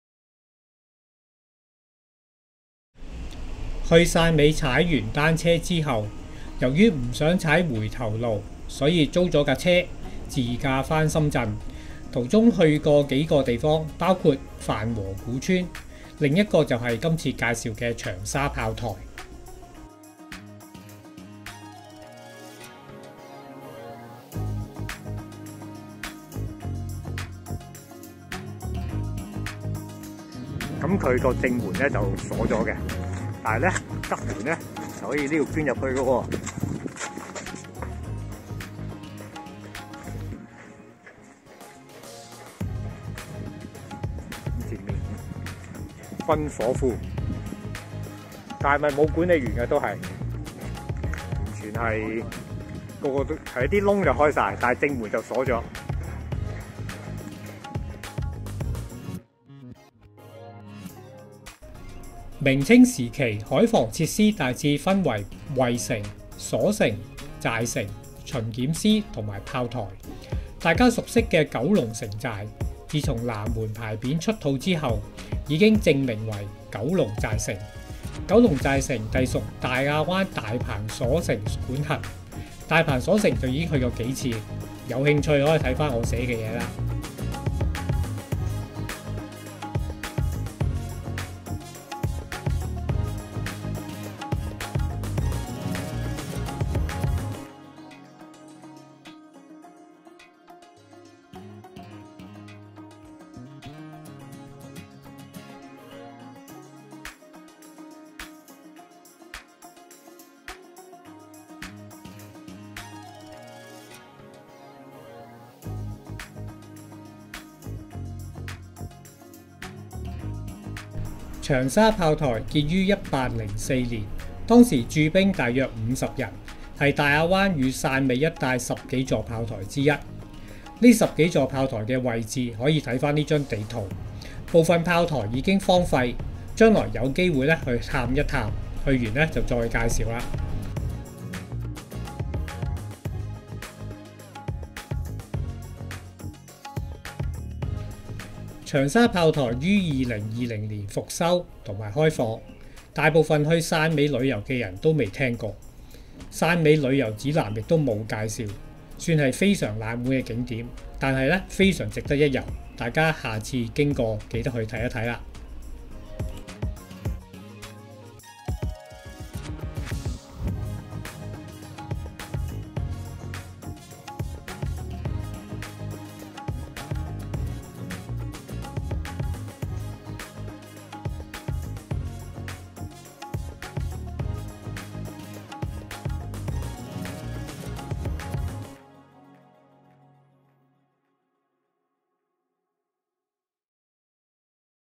長沙炮台 — 汕尾冷門景點，雖然冷門，但值得一遊【4K 中文字幕】
單車遊汕尾，去頗冷門景點「長沙炮台」，是大亞灣十幾個明清時期炮台之一。

詳盡故事 @ egg studio 網站《汕尾快車！「單車一日踩到最遠的地方」— 汕尾單車+自駕遊》

egg studio | 黃基業 (Kay Wong) |藝術 + 探險 + 人文紀實